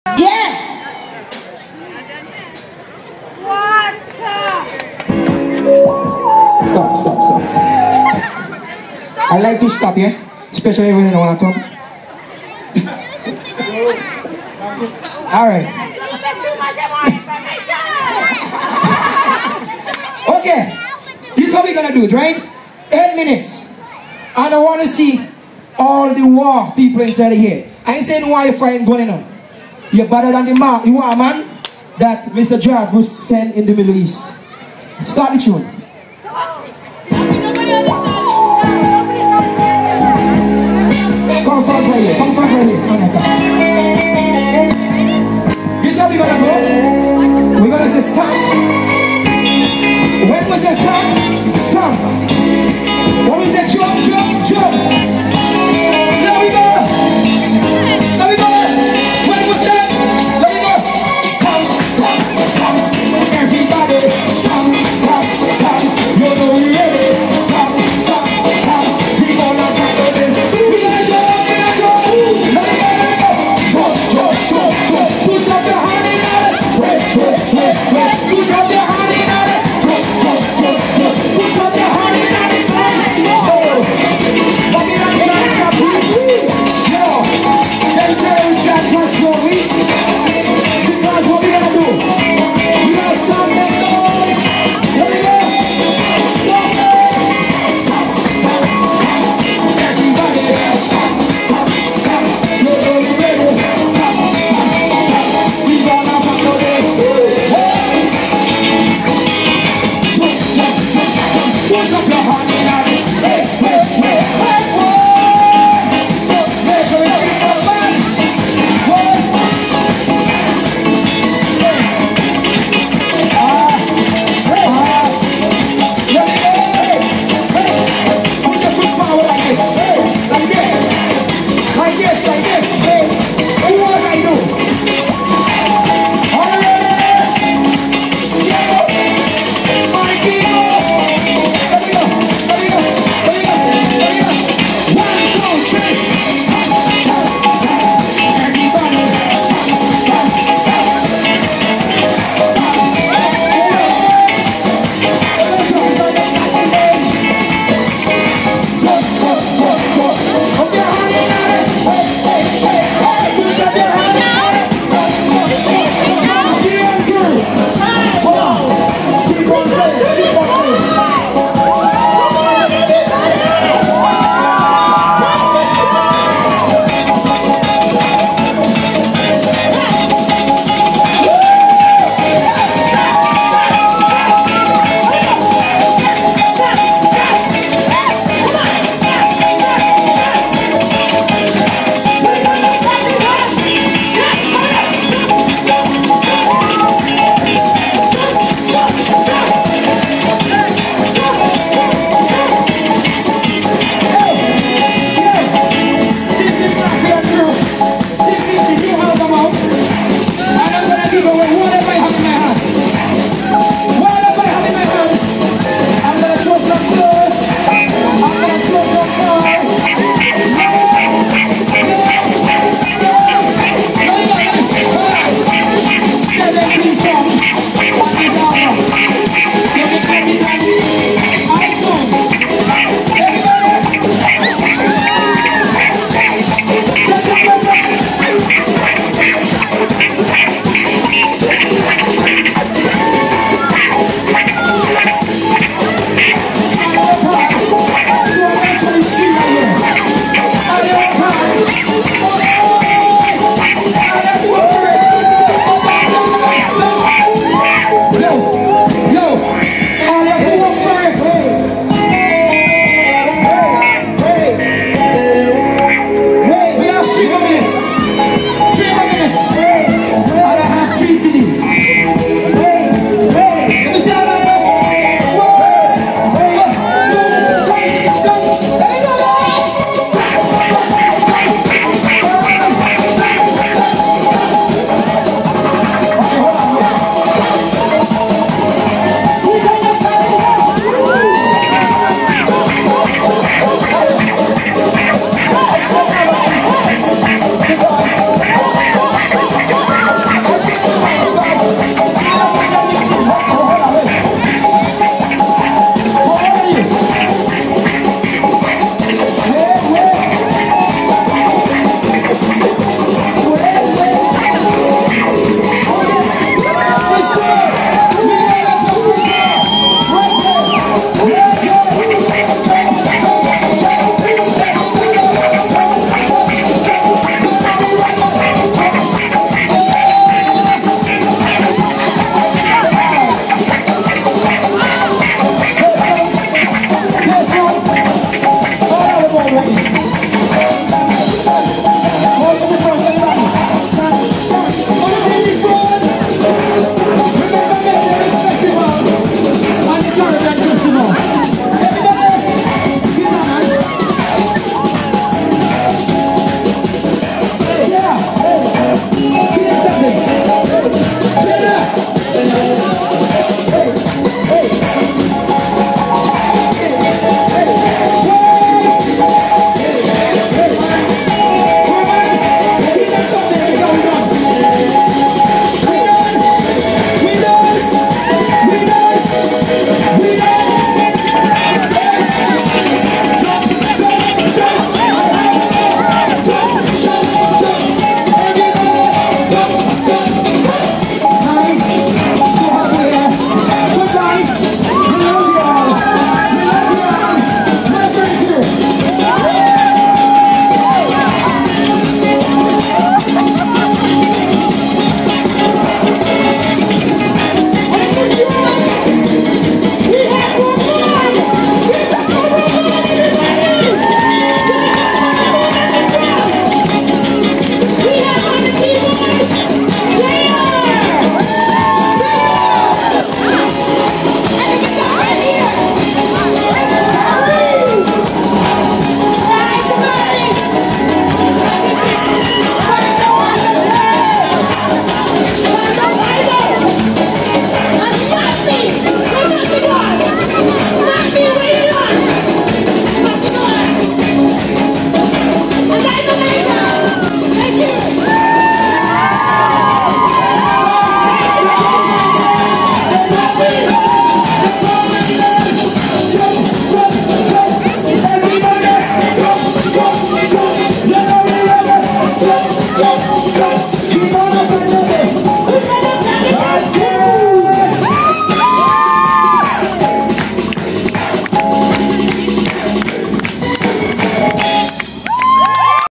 Stomp
Maffie & crew @ maple ridge Caribbean days festival.